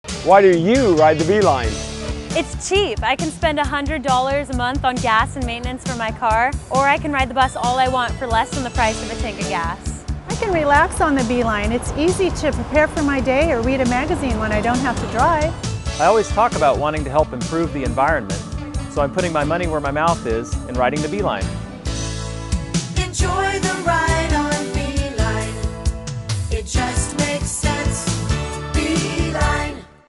B-Line Bus Ad - Testimonials
Butte Regional Transit (B-Line) TV Commercial - There's dozens of reasons to take the bus. Why do you ride the B-Line?